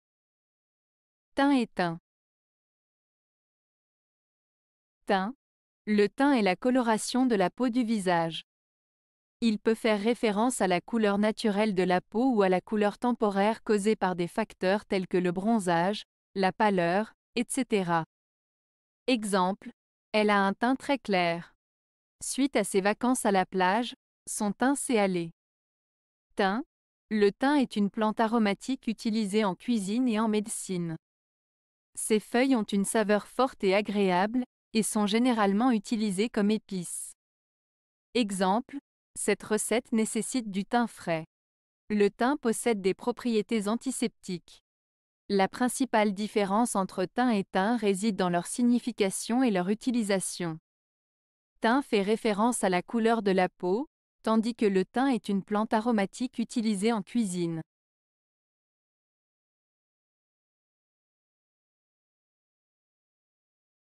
Teint et Thym (Paronymes)
Teint :
Le teint est la coloration de la peau du visage. Il peut faire référence à la couleur naturelle de la peau ou à la couleur temporaire causée par des facteurs tels que le bronzage, la pâleur, etc.

Exemples :
- Elle a un teint très clair.
- Suite à ses vacances à la plage, son teint s'est hâlé.

Thym :
Le thym est une plante aromatique utilisée en cuisine et en médecine. Ses feuilles ont une saveur forte et agréable, et sont généralement utilisées comme épice.

Exemples :
- Cette recette nécessite du thym frais.
- Le thym possède des propriétés antiseptiques.

Différence entre les deux mots :

La principale différence entre teint et thym réside dans leur signification et leur utilisation. Teint fait référence à la couleur de la peau, tandis que le thym est une plante aromatique utilisée en cuisine.